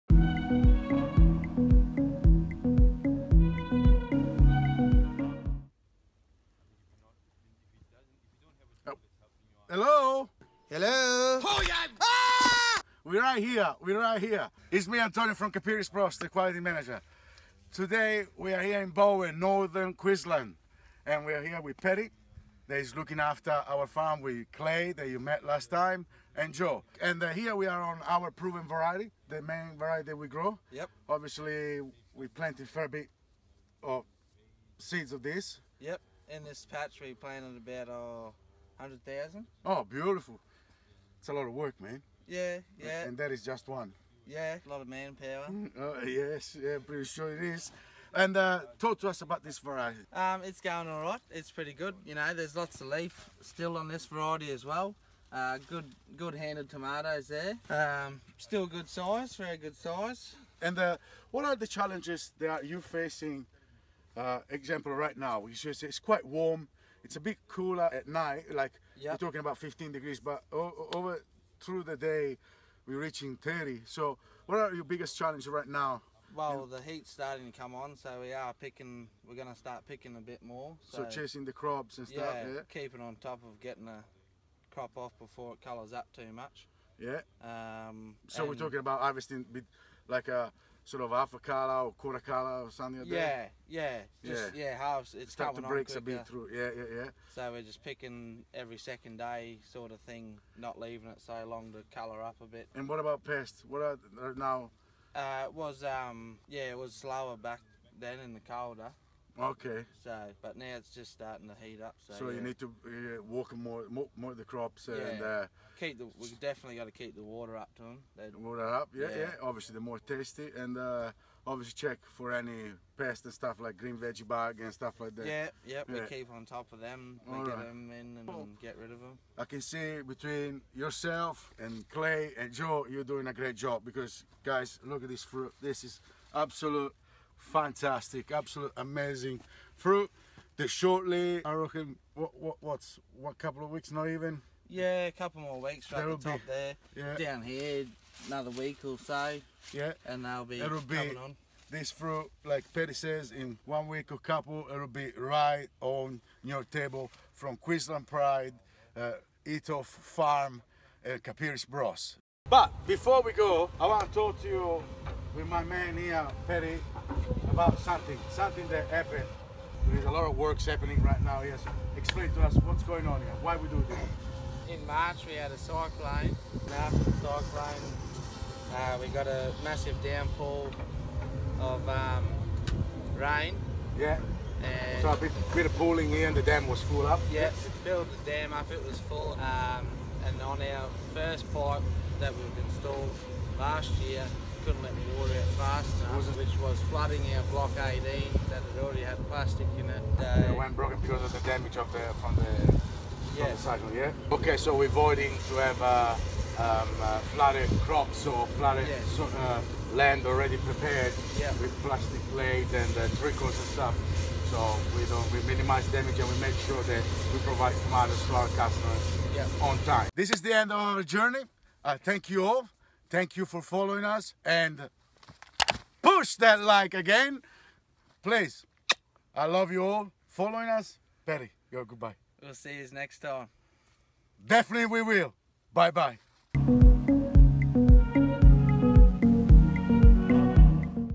Antonio's Report - QLD Pride: Pre Harvest and Dam Construction
AntoniosReport! TODAY Antonio visits BEAUTIFUL BOWEN, QUEENSLAND! 👨‍🌾🍅